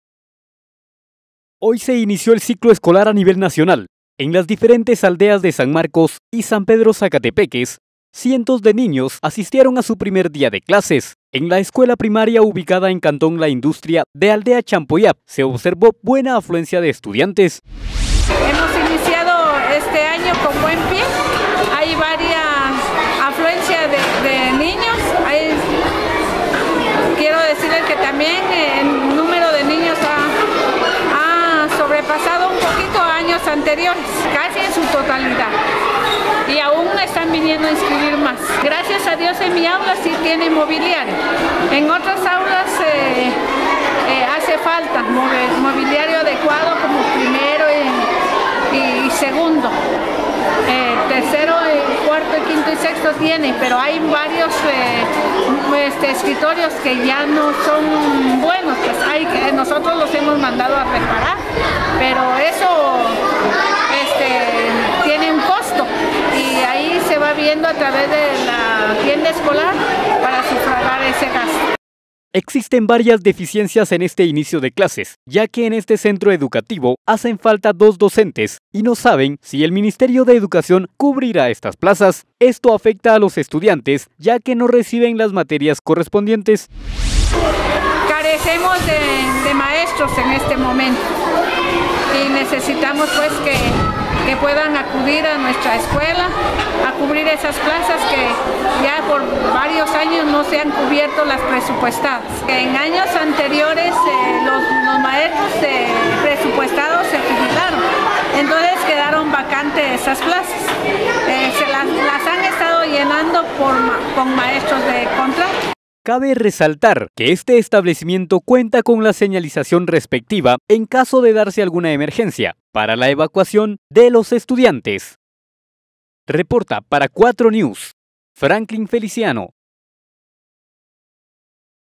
Escuela de Aldea Champollap inaugura ciclo escolar 2014.
Hoy se inició el ciclo escolar a nivel nacional; en las diferentes aldeas de San Marcos y San Pedro Sac., cientos de niños asistieron a su primer día de clases; en la escuela primaria ubicada en cantón la industria de aldea Champollap se observó buena afluencia de estudiantes.